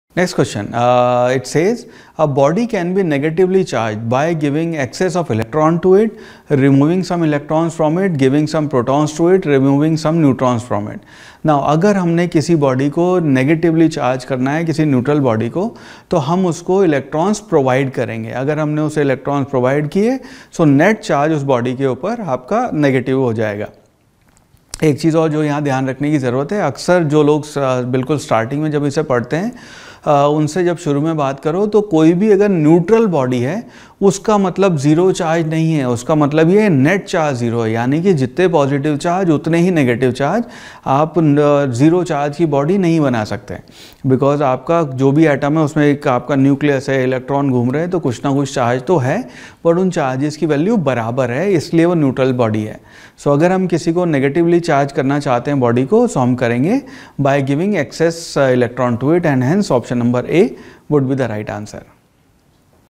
A body can be negatively charged by (a) Giving excess of electrons to it (b) Removing some electrons
A body can be negatively charged by (a) Giving excess of electrons to it (b) Removing some electrons from it (c) Giving some protons to it (d) Removing some neutrons from it